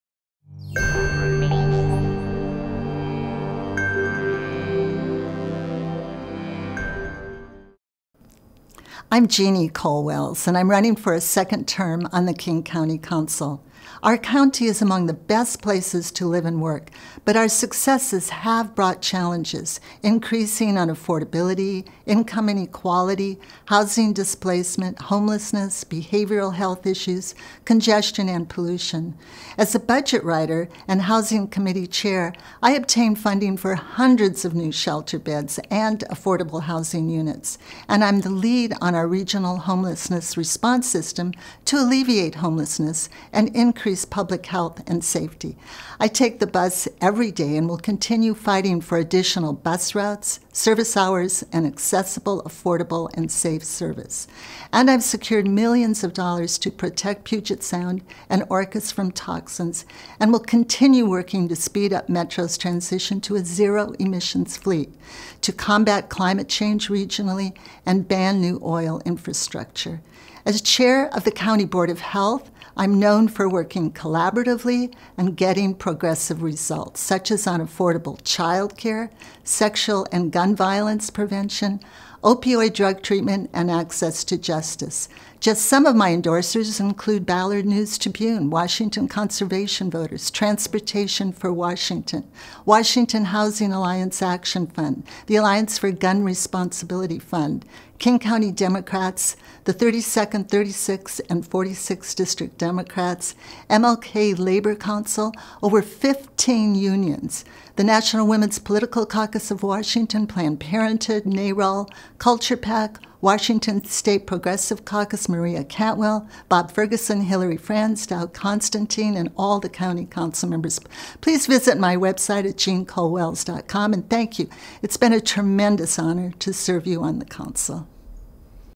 Jeanne Kohl-Welles, candidate for Metropolitan King County, Council District No. 4
Welcome to the 2019 King County General Election Video Voters' Guide, your chance to hear directly from the participants appearing on the November 5 ballot. Jeanne Kohl-Welles is running for Metropolitan King County, Council District No. 4.